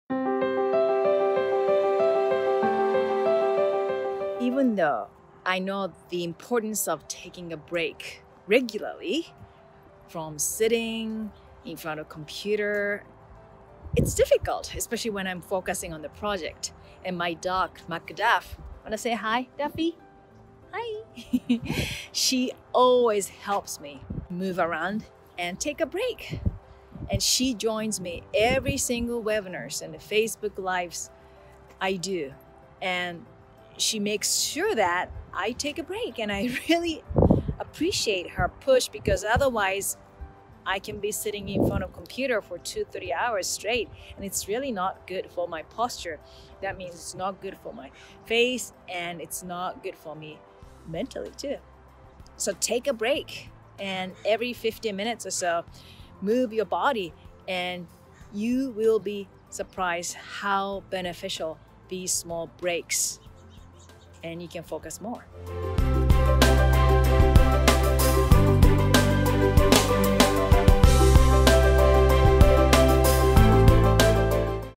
Self Care And How My Dog Is Helping Me Be More Accountable About It
Self-care is the most important way to maintain our emotional and physical health. We are so busy today that we forget to put ourselves in front of everything and everyone else.

It is no secret that finding “new” time in your day to add in a routine or some self-care is HARD.

For me, my rescue dog Duffy makes self-care much easier because he keeps me accountable for my self care routine.

I spend most of my day at my computer writing for you and to you, creating new content, and teaching my current Teacher Certification Course students their materials, so it is easy for a lot of time to go by without moving.

But Duffy is really good at motivating me to get outside and move a little! It is so much more fun knowing that I have someone to walk with outside and that I am not alone enjoying that time. It is good for him, and it is great for me!

Who is your accountability partner?

Start taking better care of yourself today by trying out our best FREE Face Yoga exercises ➡️